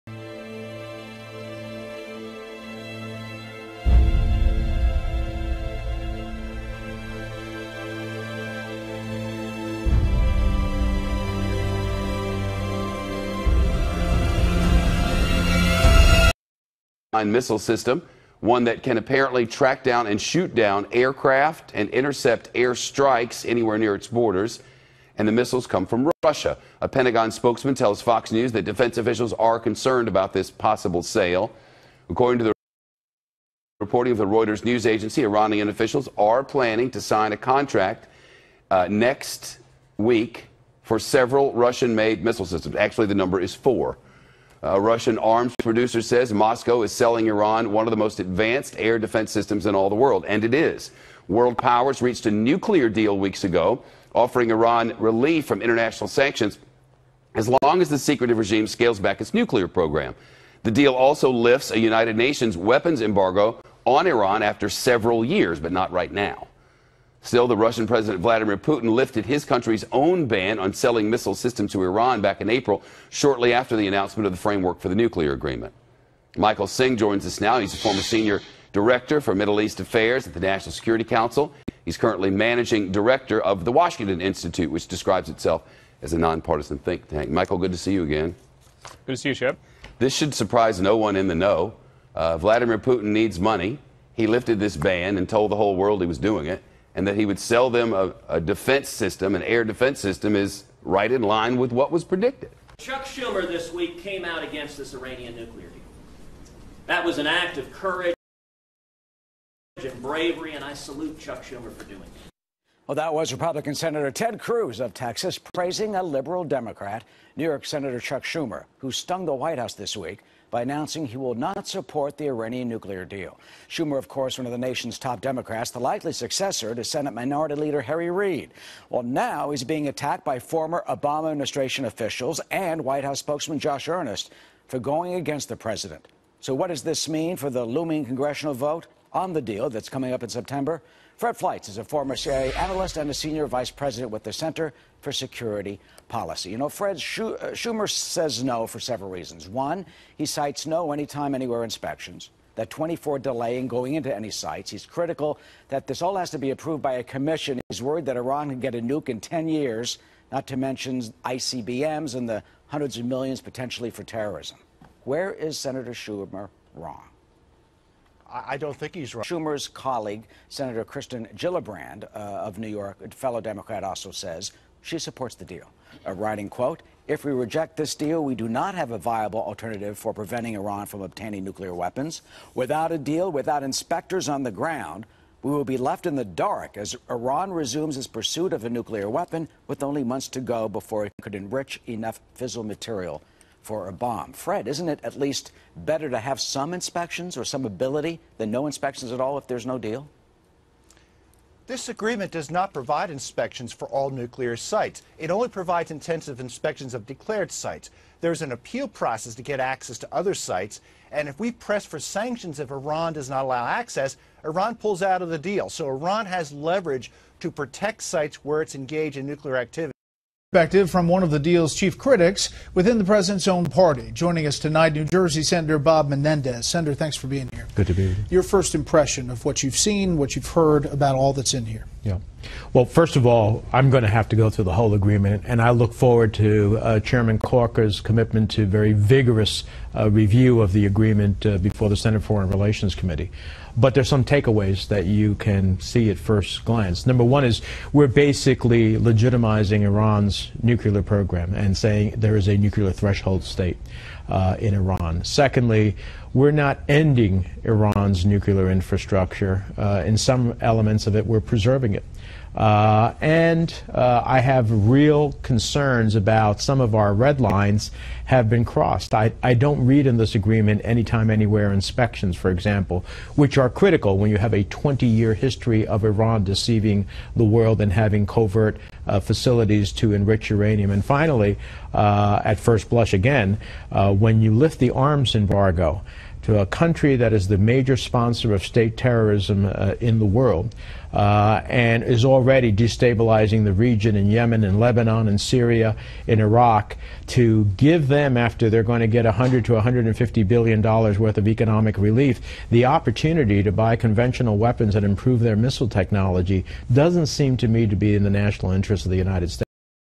Russia sells Iran Missiles! Obama's Support For the Iranian Nuke deal Dwindles!!
The air defense system is said to be the best in the world!